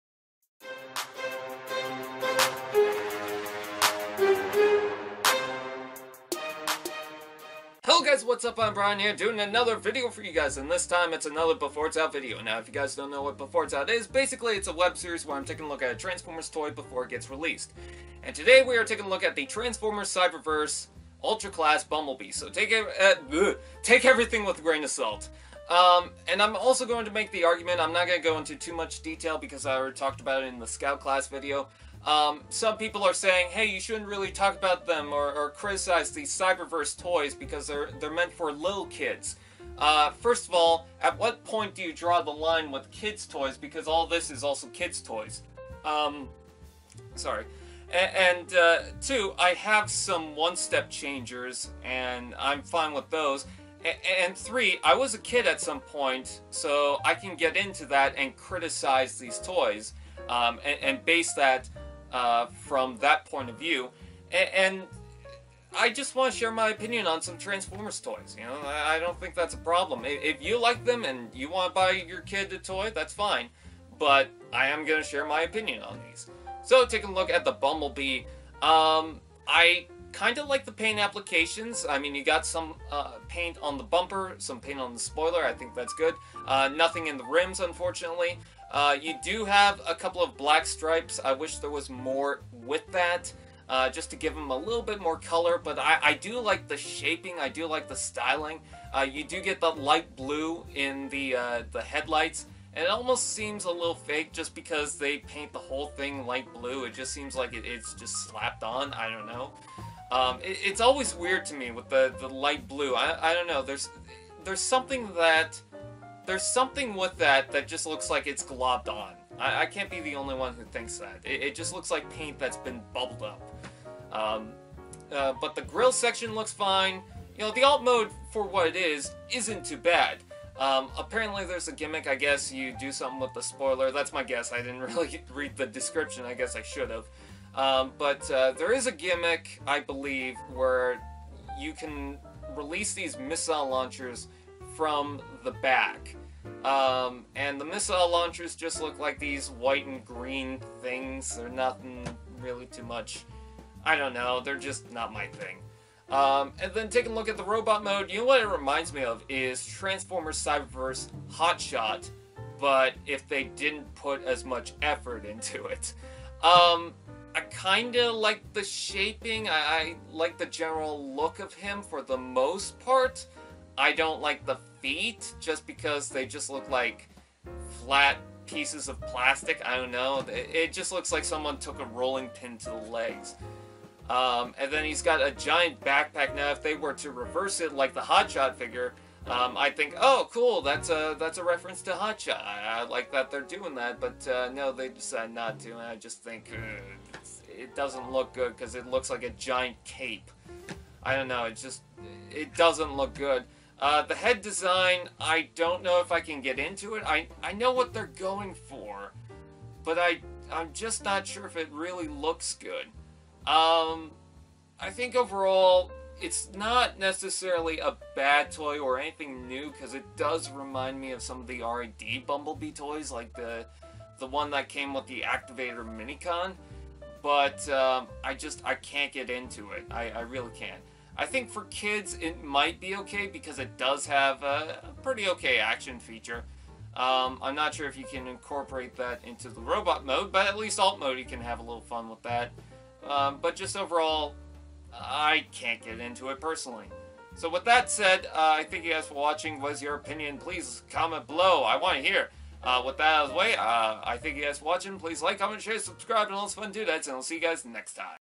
BEFORE IT'S OUT || Transformers Cyberverse Ultra Class Bumblebee [Photo Pre-Review]
This video was filmed on February 26th 2018, information and opinions judging this toy may be altered once the figure is released, take everything with a grain of salt but don't get too salty.

Welcome to another BEFORE IT'S OUT, a series where I take a look at a figure before it's released as a pre-review. Today's topic is Transformers Cyberverse Ultra Class Bumblebee.